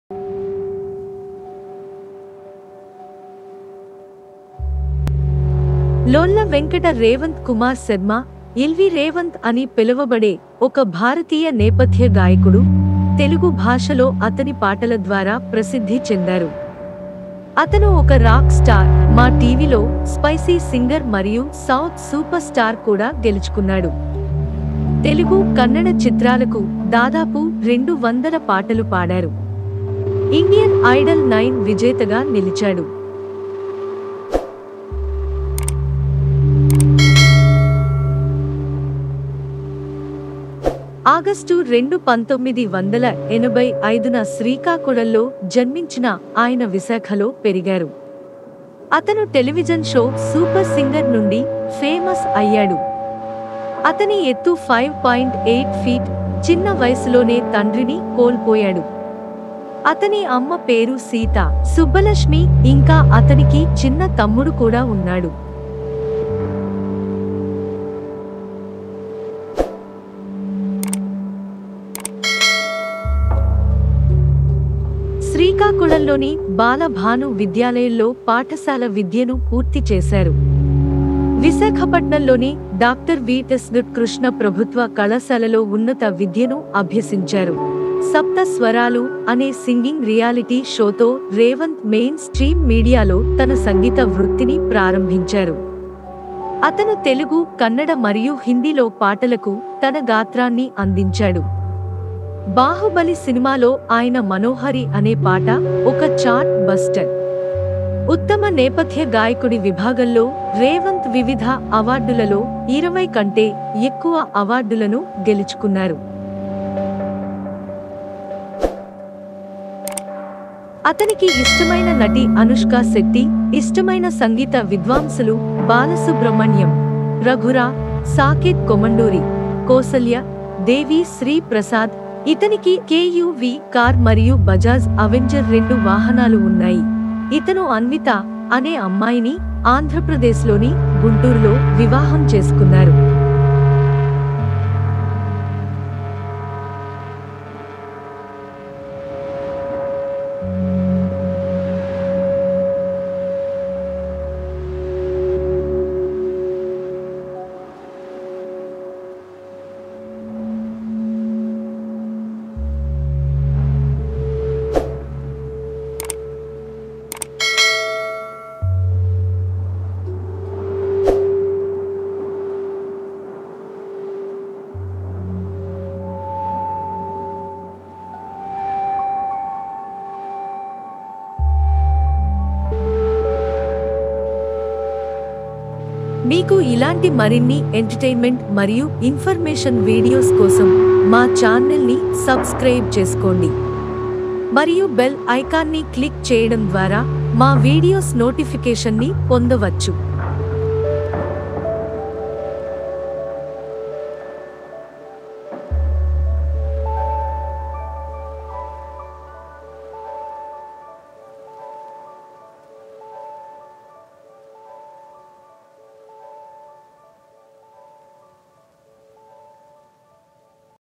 Bigg Boss 6 Telugu Contestant LV Revanth Full Biography, Wife, Awards, Girlfriend, Facts
Bigg Boss 6 Telugu Contestant Lolla Venkata Revanth Kumar Sharma Full Biography, Wife, Awards, Girlfriend, Facts

Watch - ఇలాంటి  10 మంది వ్యక్తులు నిజంగా ఉన్నారని మీరు నమ్ముతారా | 10  People You Won’t Believe They Exist --